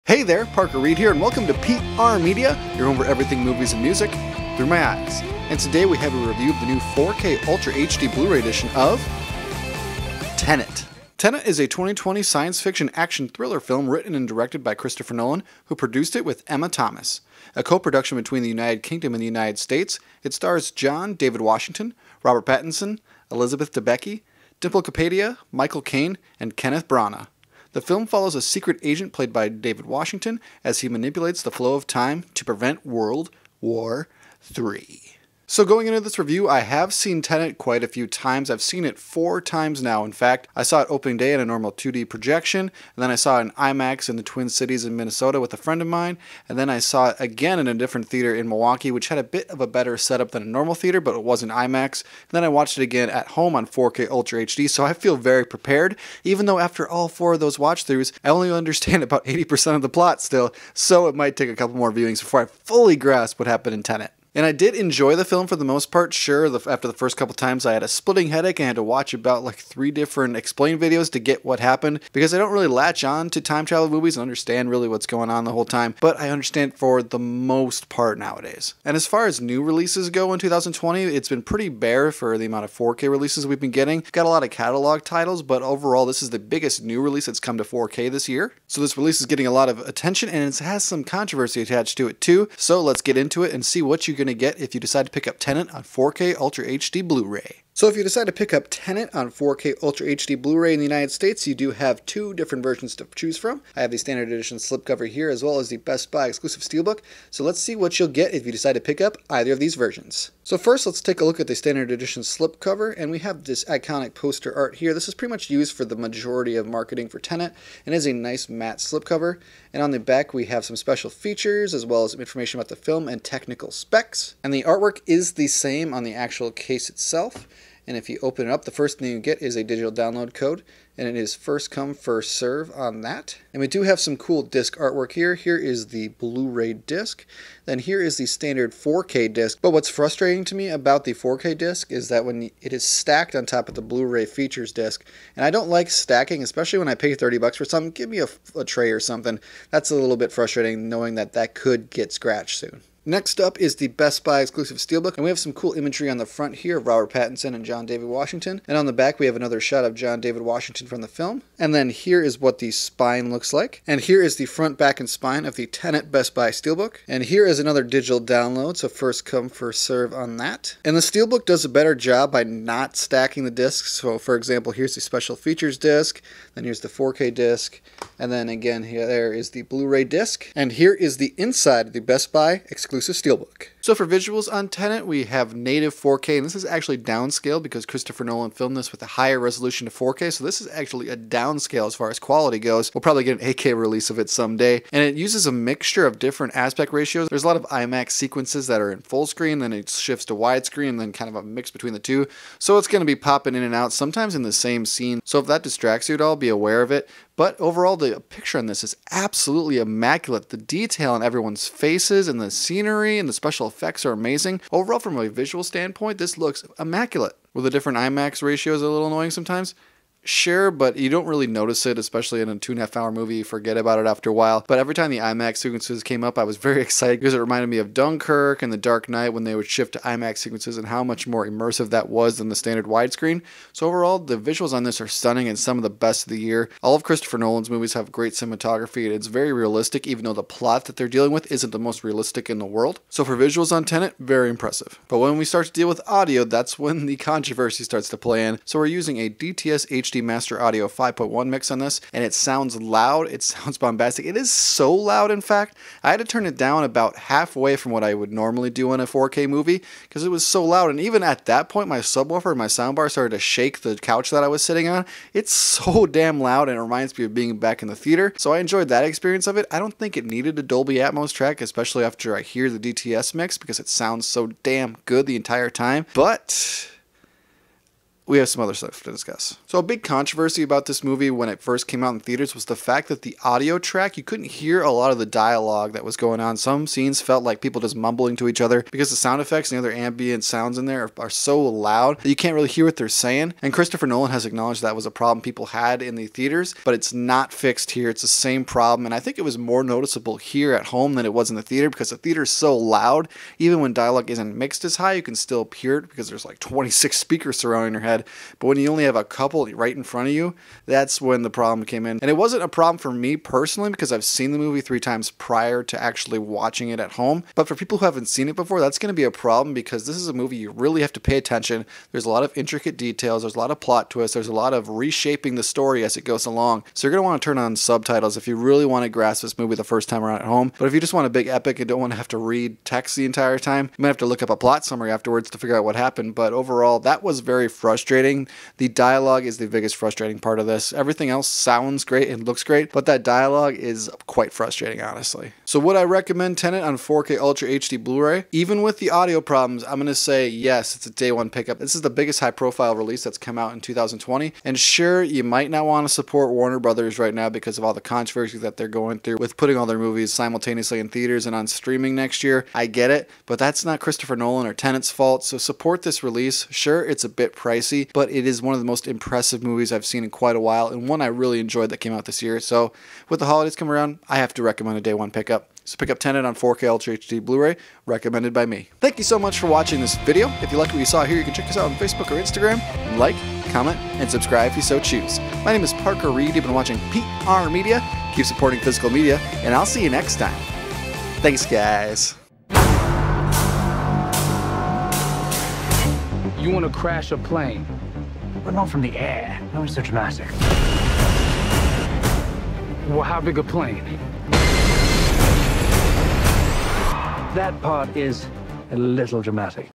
TENET 4K Ultra HD Blu-Ray REVIEW
Here I review the new 4K Ultra HD Blu-Ray edition of Christopher Nolan's, "Tenet." Are the audio issues fixed? Are the IMAX sequences included? Is it worth a pickup? Let's find out...